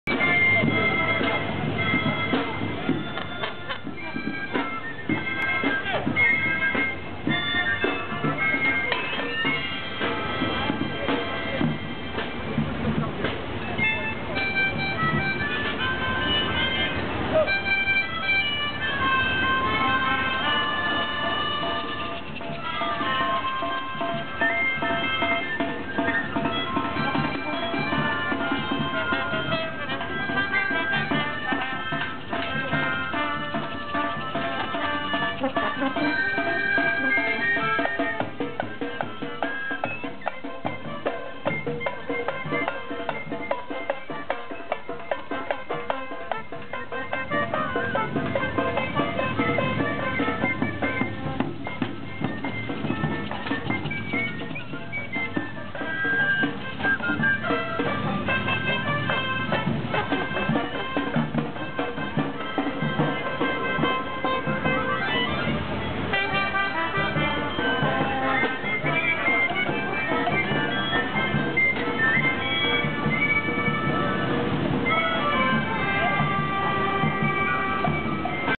Cyclophonica Paisagem Sonora Carioca , Concerto 12, Sons da Tijuca
Em 15 de maio de 2011 a Cyclophonica percorreu o bairro da Tijuca tocando nas principais praças do bairro e nas ruas.
O projeto Cyclophonica Paisagem Sonora Carioca tem o patrocínio da Oi e Secretaria de Cultura do Estado do Rio de Janeiro, com apoio da Oi Futuro
Video feito por Zenon Silveira